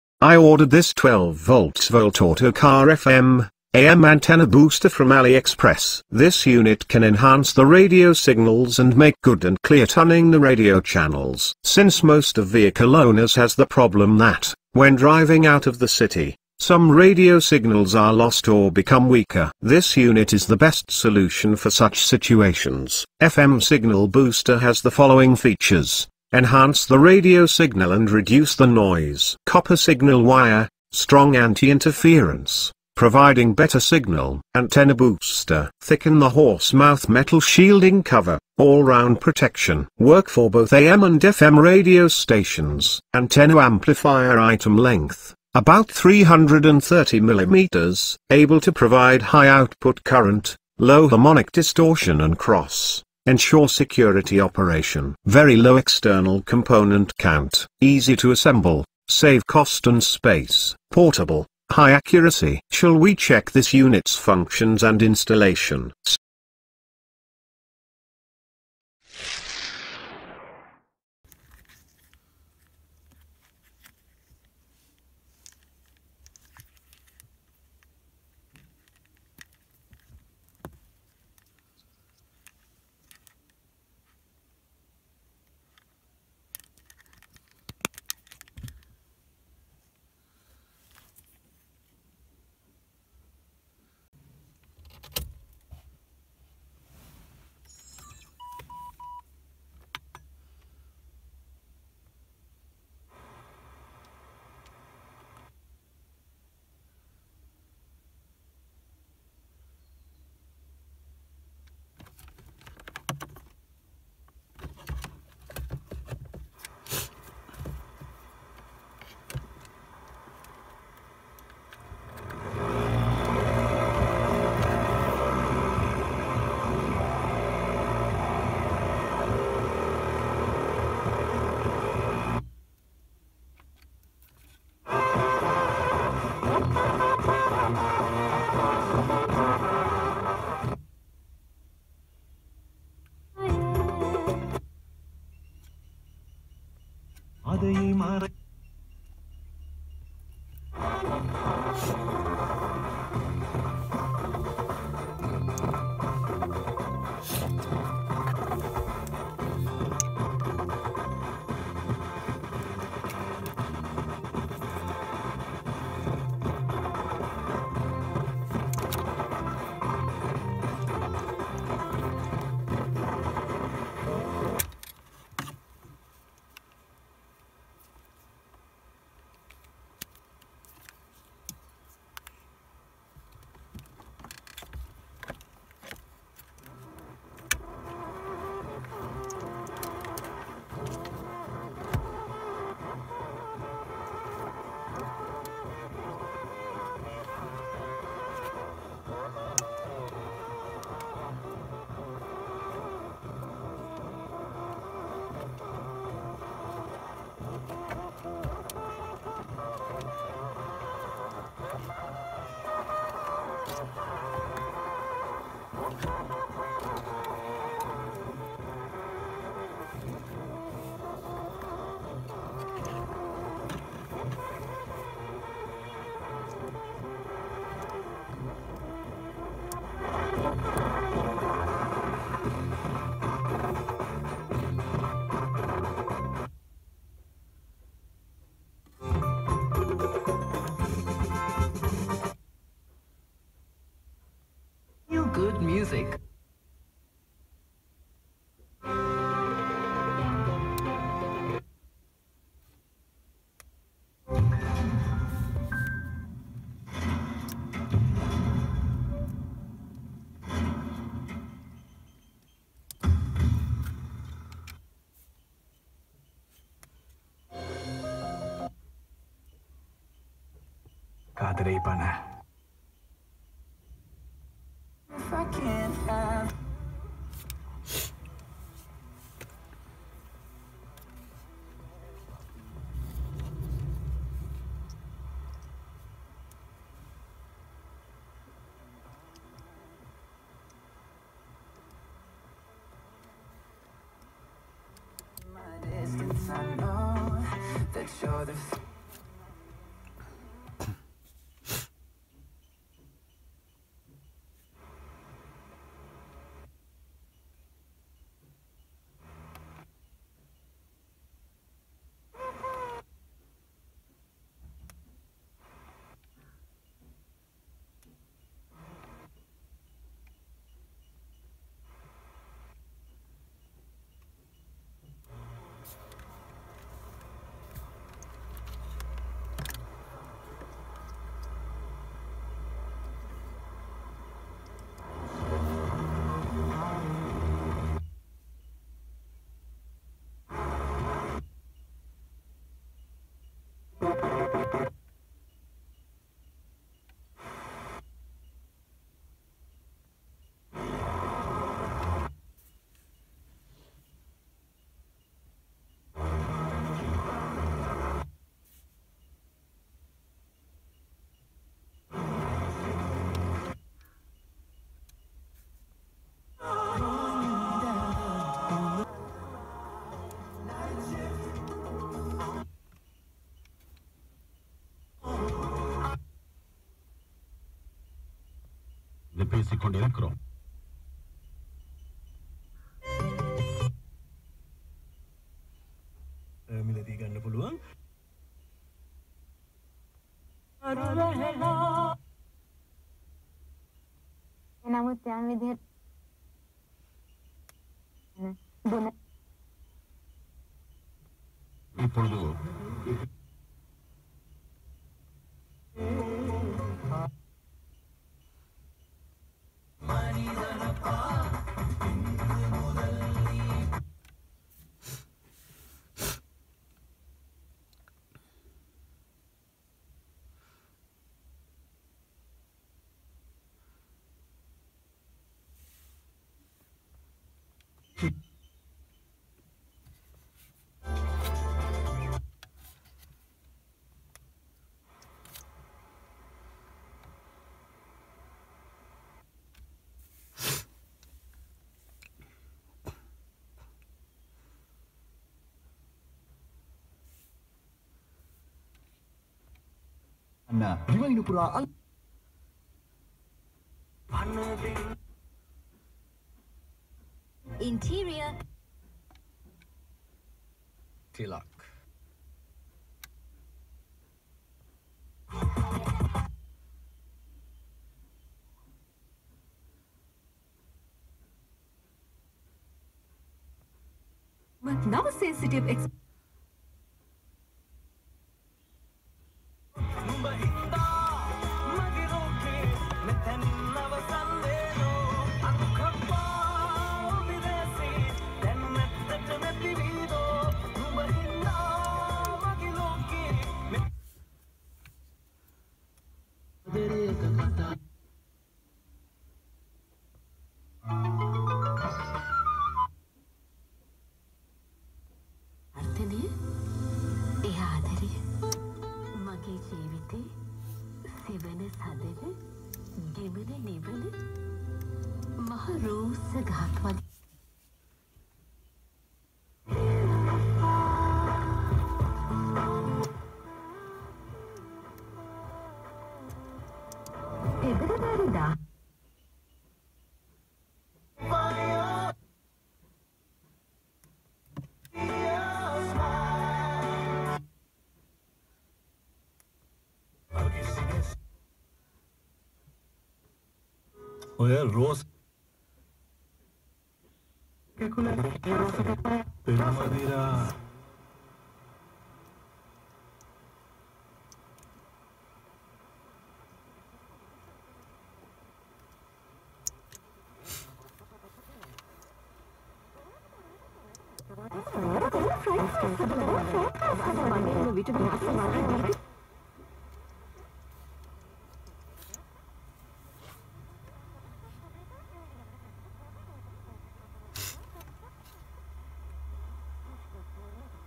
Car FM/AM Antenna Booster Installation and Testing
❤️‍🔥❤️‍🔥 Link to purchase

Nowadays there are many ways to entertain the peoples sometimes it may be internet, mobile phones, ipads, and tablets. But most of people still believe that and love to listen FM# radio channels. Especially when you are driving a long journey, no one can make you happy than a FM radio.
But the problem is when you go far from the city and farther from the radio signal trovers, you are not able to listen to the radio clearly. As you know if it is a TV at home, we use a TV booster. As same as this, you can use an FM booster as well. Therefore I search to purchase FM booster for my vehicle and I ordered this car FM/AM antenna booster from aliexpress. This booster is most effective for FM frequency and working with 12 volts. This booster is a powerful one. However, the area i am checking this booster has good FM radio signals. Therefore, you may not feel a big difference.
but when you drive no signal or signal week area, you will feel that you are still at home. :-)